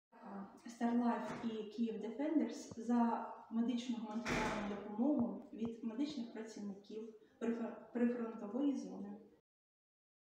Thanks from the medical workers of the front zone for humanitarian aid.
🇺🇦 𝗧𝗼𝗱𝗮𝘆 𝗶𝗻 𝗨𝗸𝗿𝗮𝗶𝗻𝗲 𝘄𝗲 𝗵𝗮𝘃𝗲 𝗳𝗼𝗹𝗹𝗼𝘄𝗶𝗻𝗴 𝗻𝗲𝗲𝗱𝘀:
- Hygienic products
- Camping equipment: sleeping bags, tents, burners, walkie-talkies, etc